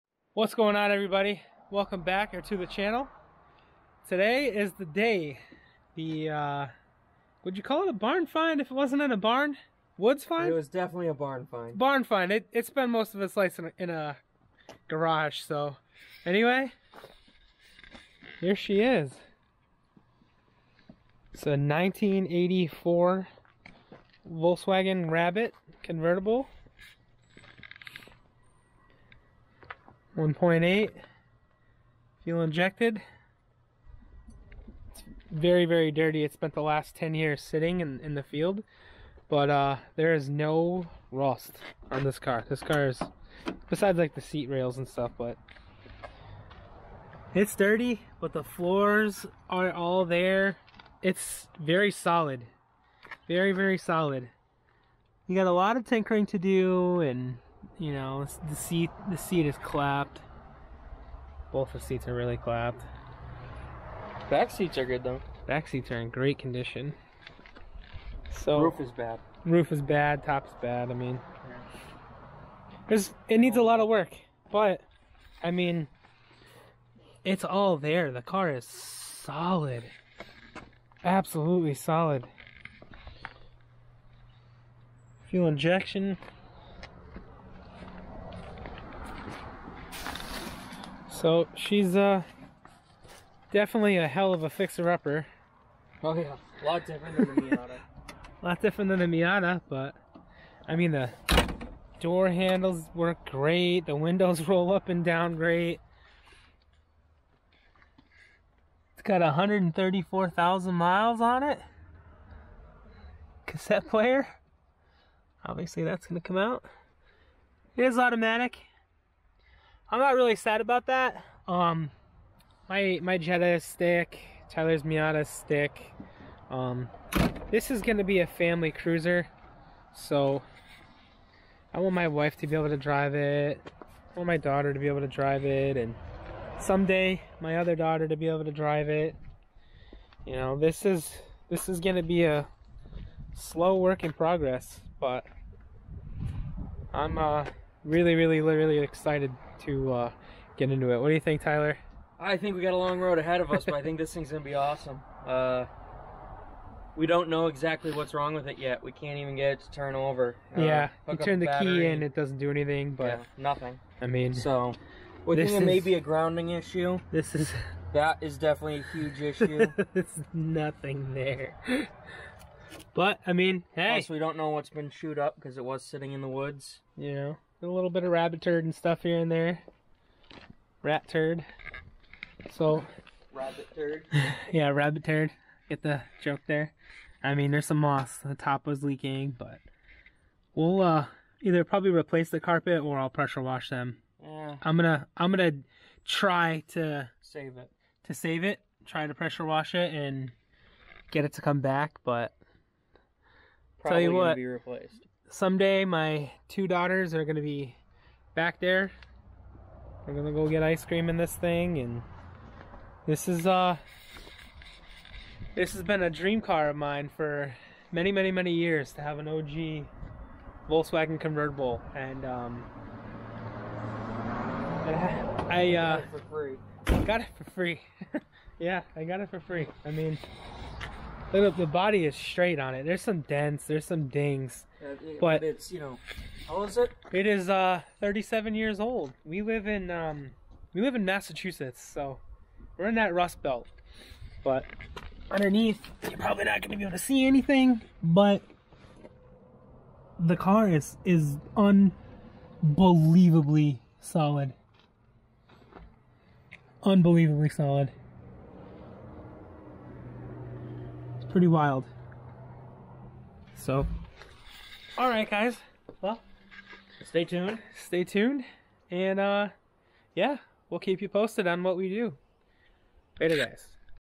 Is It a "BARN FIND" If we didnt Get It Out Of a Barn?!
We got the car! Justin's new "Barn" Find well... field find? this car has been garaged nearly its whole life theres basically NO rust!